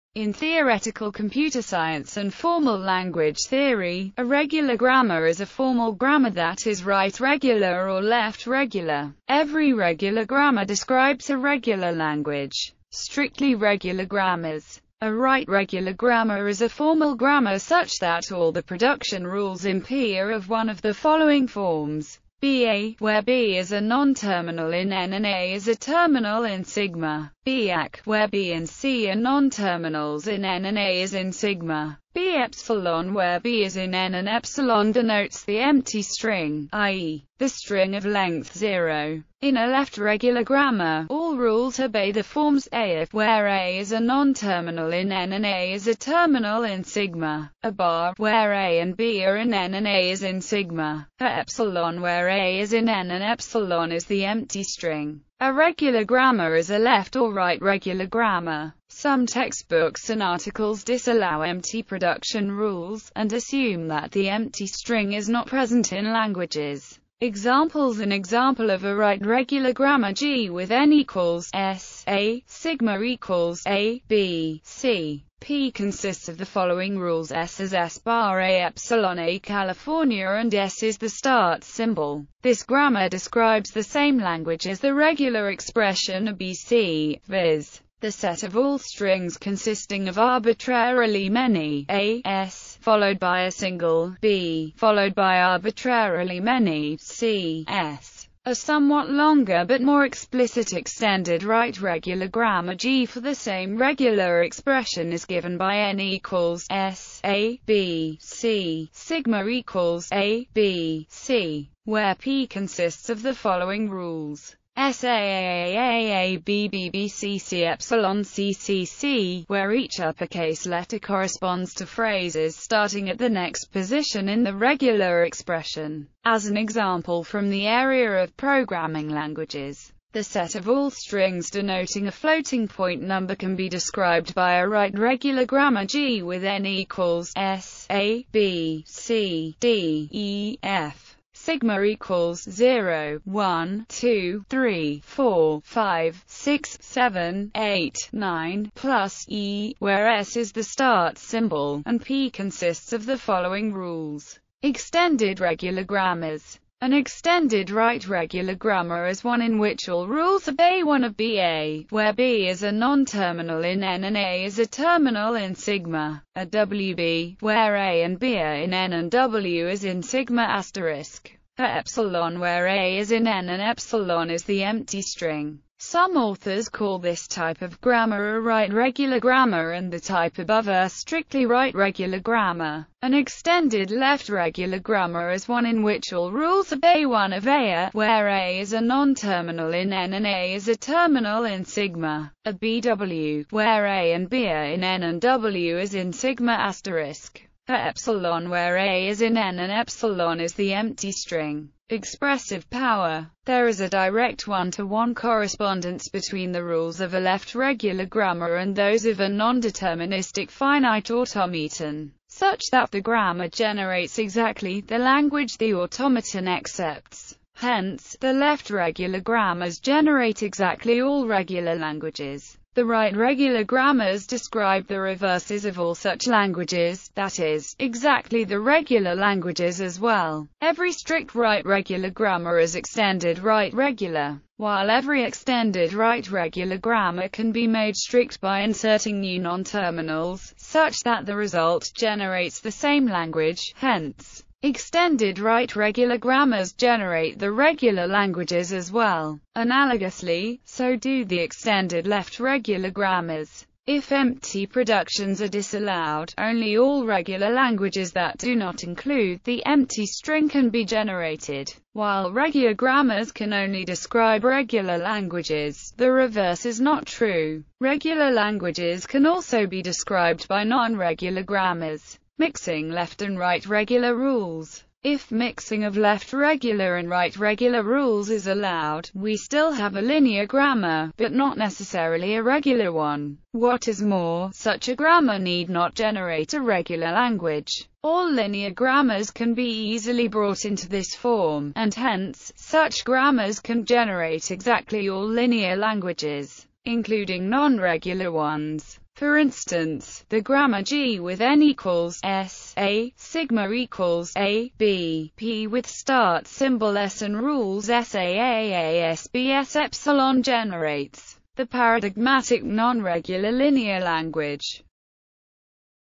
Regular grammar
In theoretical computer science and formal language theory, a regular grammar is a formal grammar that is right-regular or left-regular.Every regular grammar describes a regular language.